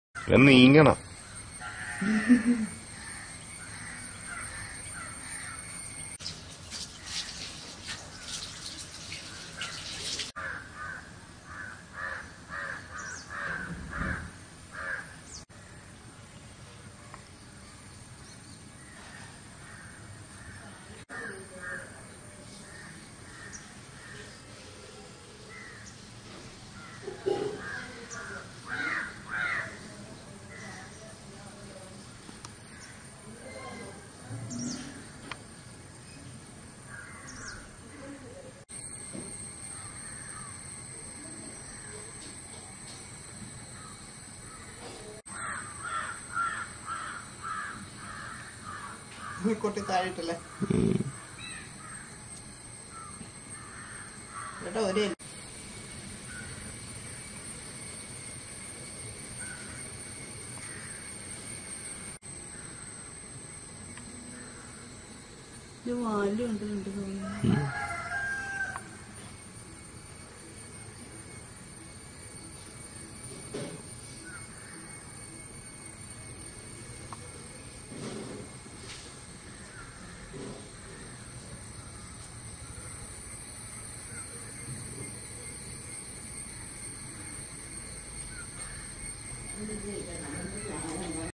Green insect, Kerala
This is an unusual green insect spotted by my 3 year old son at Alappuzha, Kerala. It was small in size (3 inch in length) and resembled in looks like a toy train!. It had an unusual shiny green skin 6 small black horns and two black dot eyes.Only zoologists can say authoritatively on this rare breed insect.